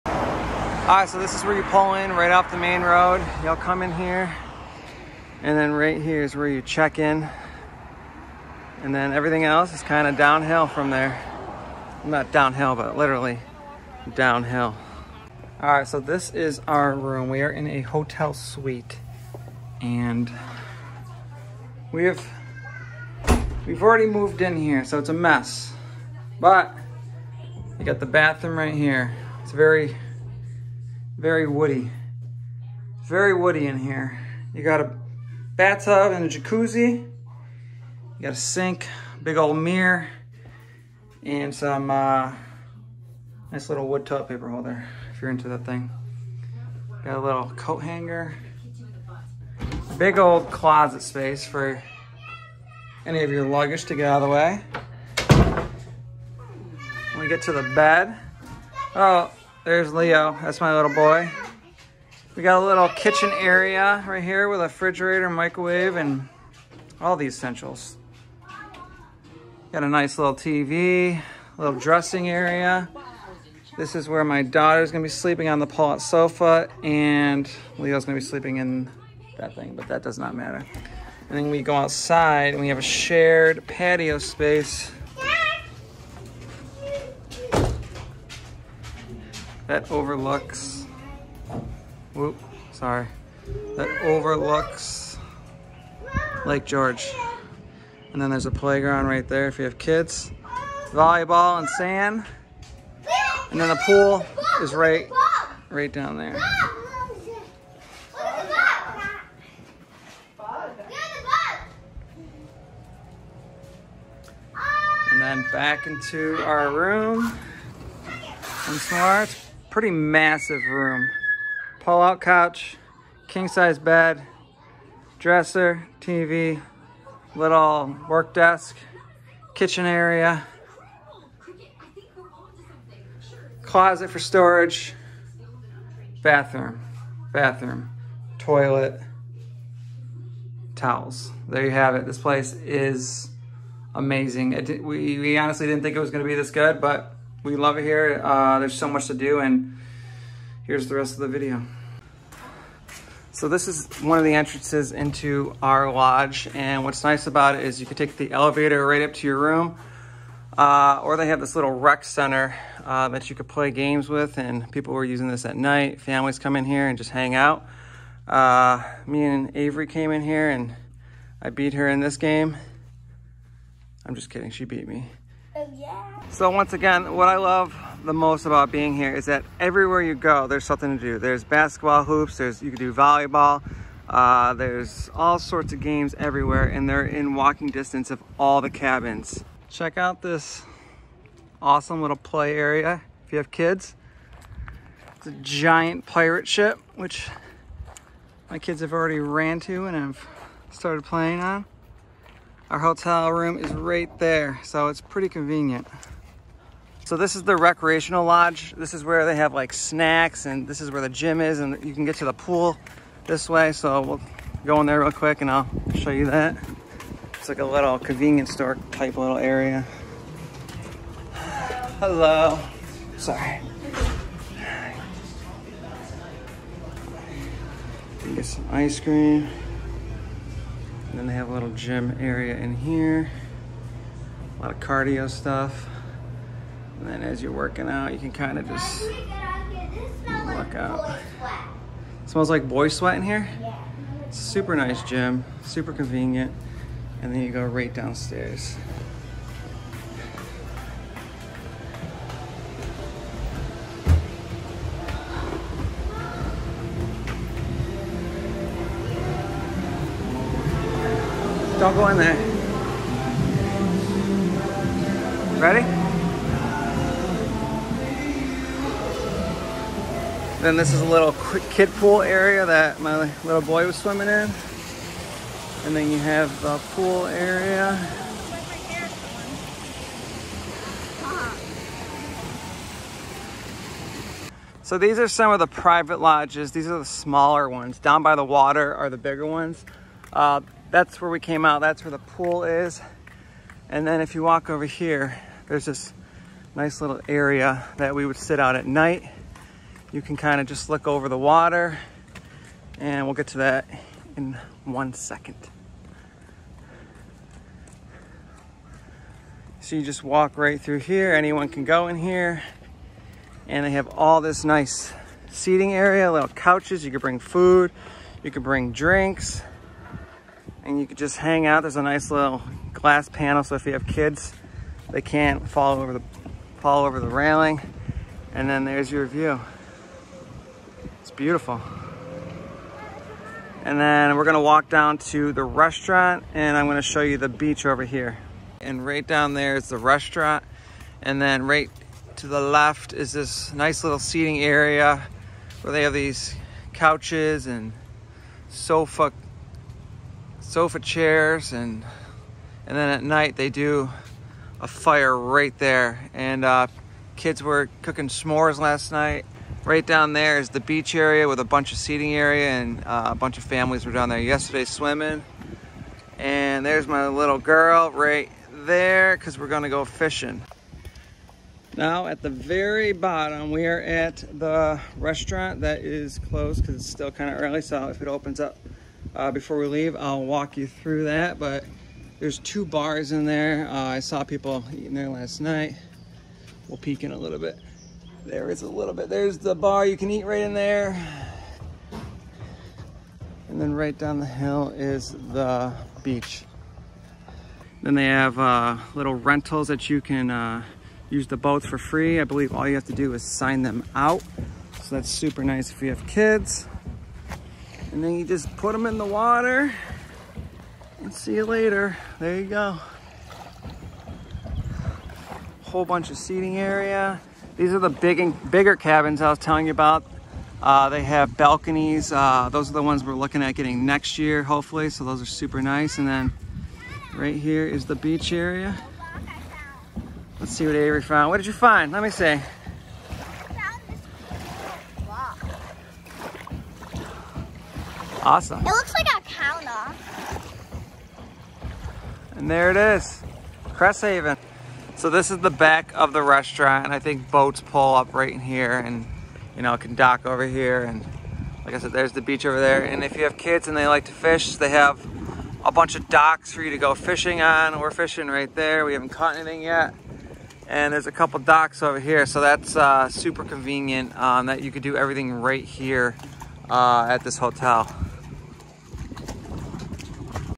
Unwind in Serenity at The Lodges at Cresthaven: A Lake George Getaway Review
Looking for a relaxing vacation spot to unwind and rejuvenate? Look no further than The Lodges at Cresthaven, nestled on the shores of the pristine Lake George in upstate New York.

In this video, we take you on a virtual tour of The Lodges at Cresthaven, showcasing its stunning location, luxurious accommodations, and first-class amenities. Whether you're planning a romantic getaway or a family vacation, The Lodges at Cresthaven offers something for everyone.

You'll be amazed by the spacious and well-appointed lodges, each designed to offer the perfect blend of comfort and elegance. From cozy one-bedroom units to spacious four-bedroom lodges, there's a perfect accommodation for every group size and preference.

Explore the resort's various facilities, such as the sandy beach, the outdoor heated pool, and the fitness center. Take in the breathtaking views of the lake, while indulging in a variety of water sports and activities such as kayaking, paddleboarding, and fishing.

During your stay, indulge in delicious meals and cocktails at The Boathouse Restaurant, featuring fresh seafood and local ingredients. Or head to the Lake George village, just a short drive away, for shopping, dining, and exploring the rich history of the region.

So pack your bags and come along with us to experience the ultimate getaway at The Lodges at Cresthaven, where every moment is a cherished memory.